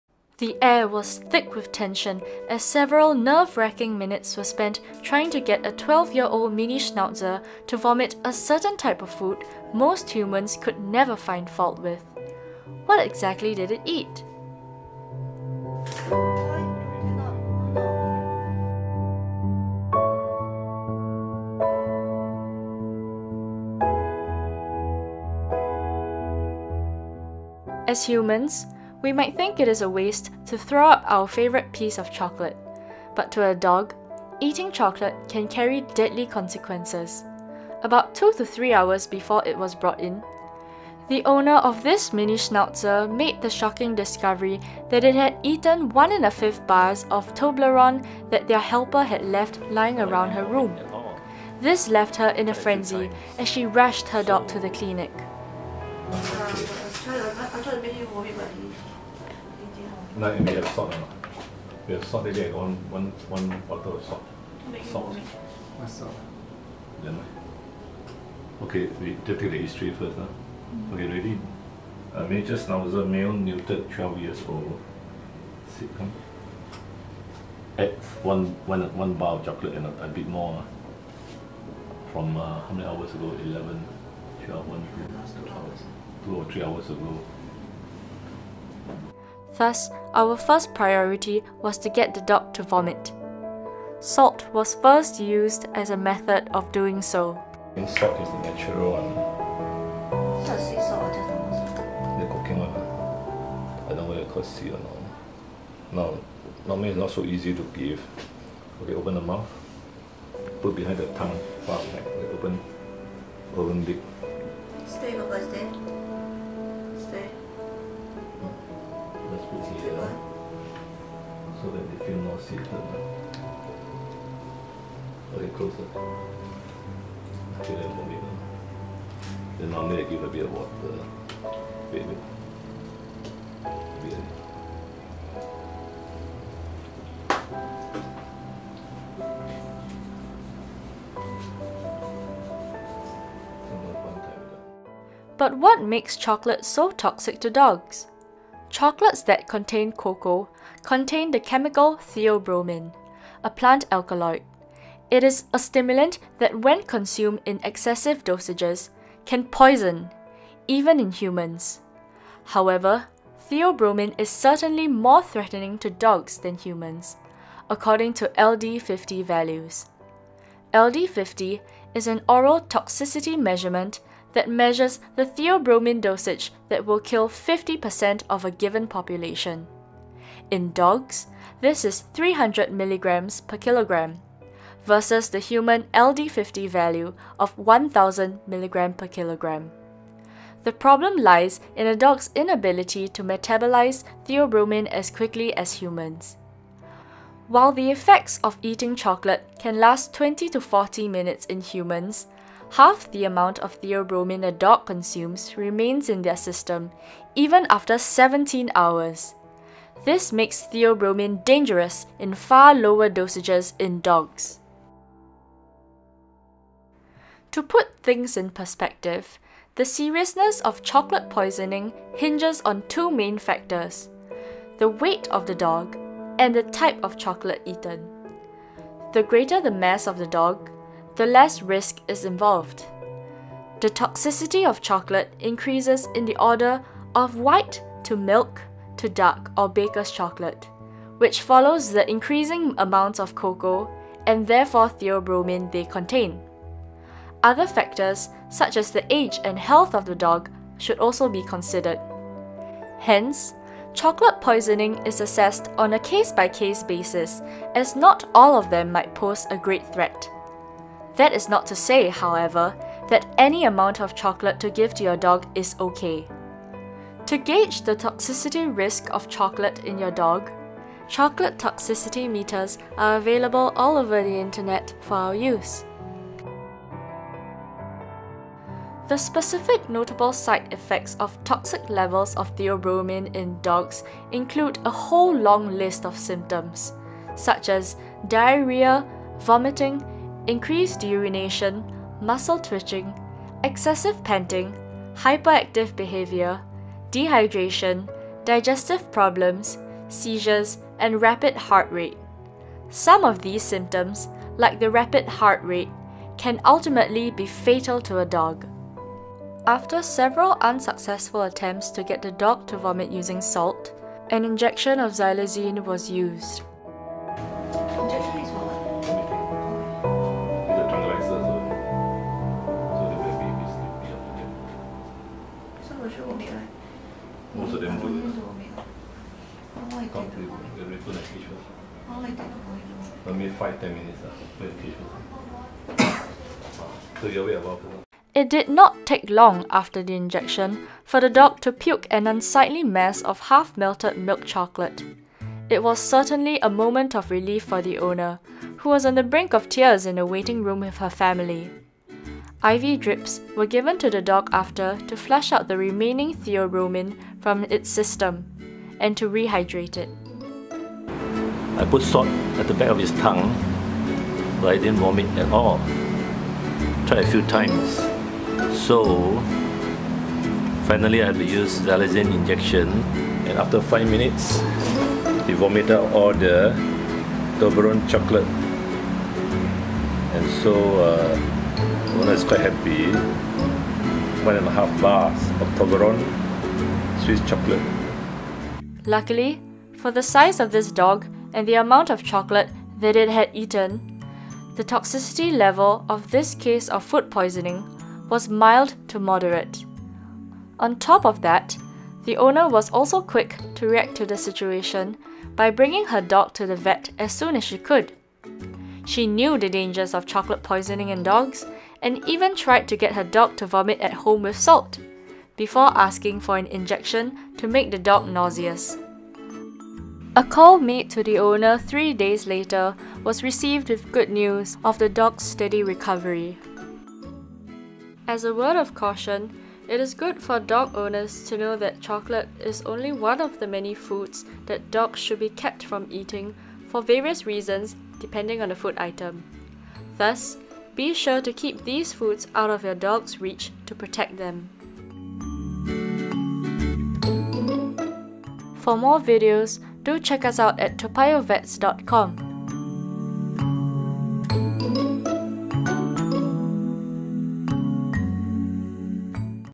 Dogs and Chocolate A deadly combination
Dogs are opportunistic and ate a bar and fifth of Toberlone chocolates. Salt on the back of the tongue did not induce vomiting in this Schnauzer. 2% xylazine 0.6 ml IM caused vomiing. An IV drip was also given. The dog is OK when I followed up 5 days later

LD means Lethal Dose.